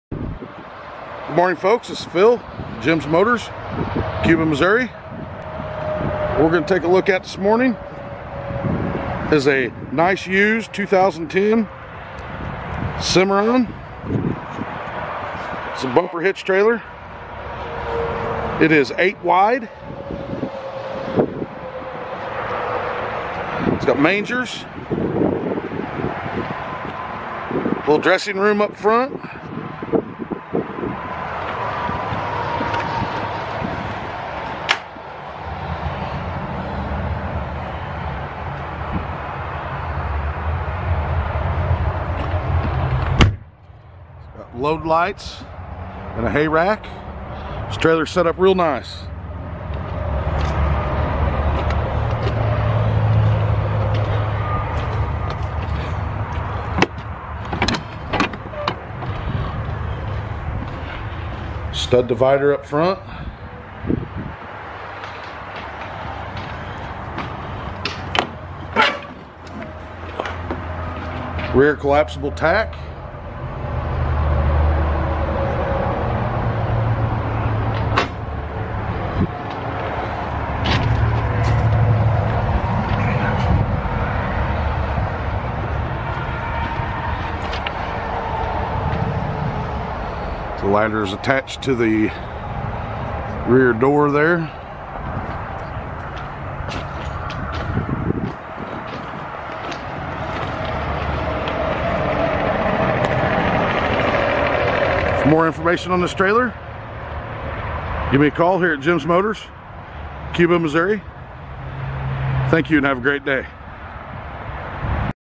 NICE 2010 Cimarron 8 wide mangers hay rack
just in on trade is a NICE 2010 Cimarron 8 wide mangers hay rack this trailer is set up nice and is ready to pull..call or text phil or email
we take trades and offer financing and delivery ..